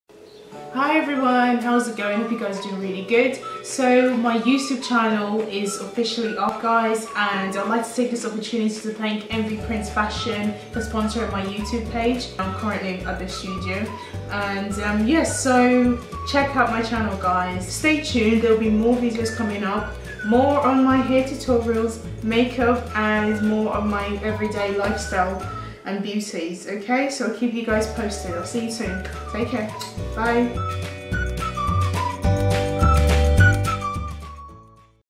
Yay, I'm sponsored!
Hi guys!!
I would like to take this opportunity to express my appreciation for the opportunity to meet and greet M.V.Prints Fashion. I would like to thank them for sponsoring my YouTube channel...
I am very excited to be representing such a wonderful company with a great taste in African Prints, so modern and beautiful.

Here at M.V.Prints Fashion they bring a taste of Africa to the UK and worldwide by shaking up the dark and bland colour palette with some nice, bright patterns - whist not being to invasive. They want the general public to want to wear their clothes without feeling like they’re is too flamboyant; where they would stick out and look silly. So, they are mixing the styles together to create something that hasn’t been done before. Something that looks modern yet different to most.

I look forward to continuing this relationship and heres to future meetings with such an amazing company.

They will be launching in Autumn 2017.
Check them out @M.V.Prints

Details of my look

*BROWS*
Vital waterproof eyebrow pencil at local cosmetic shop

*EYES*
L.A girl PRO. Conceal high definition concealer ‘Yellow corrector’

*Concealer*
NC45 Studio finish

*LIPS*
NYX PROFESSIONAL Soft matt lip cream ‘Monte carlo’

*GLOW*
Sleek form contouring & blush palate

*SKIN*
 Mayberline New York ‘New Baby Skin’ instant pore eraser
 L.A. girl Pro setting spray

**Make-up brush**
 I used- Realtechniques brushes